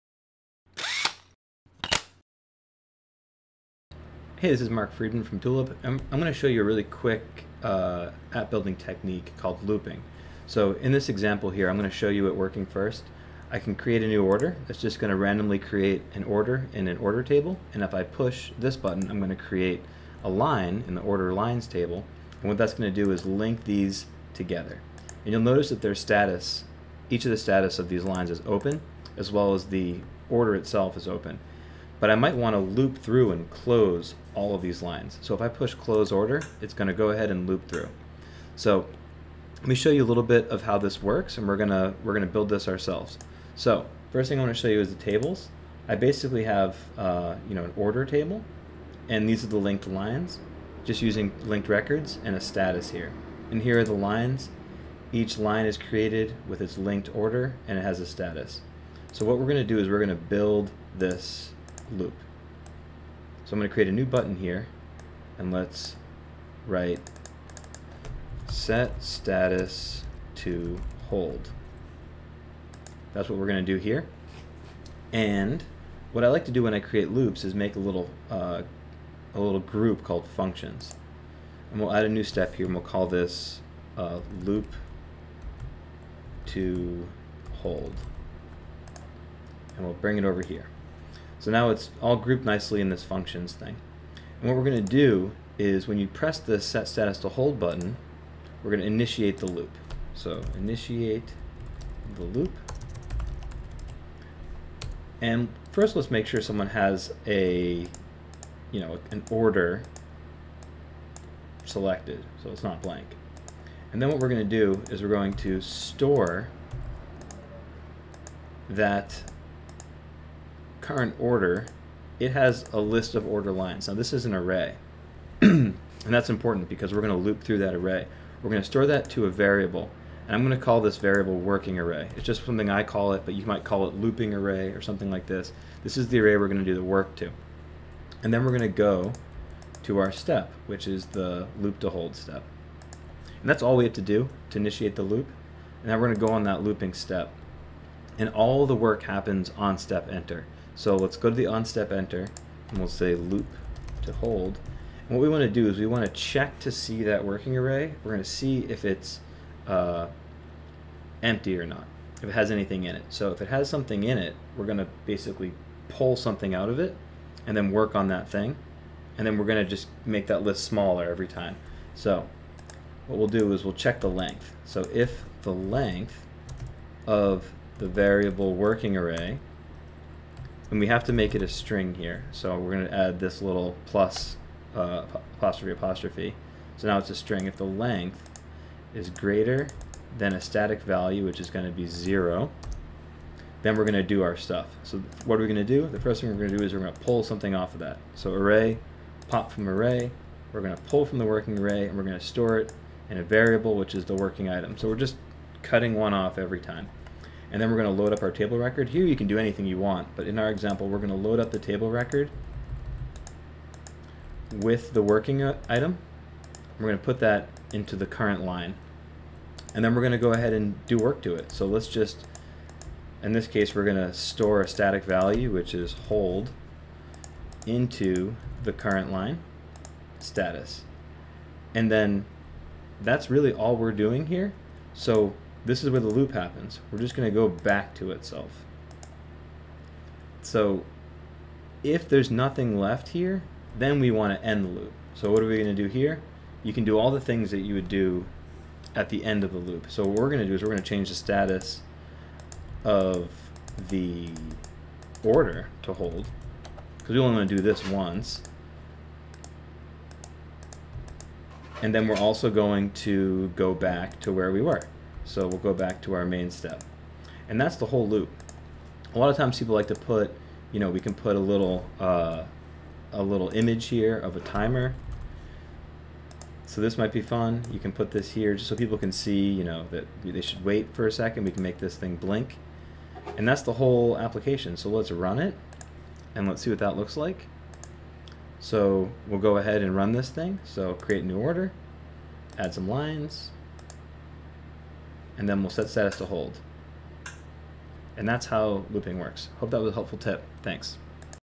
Quick tips - Looping
Learn how to use looping in your Tulip Apps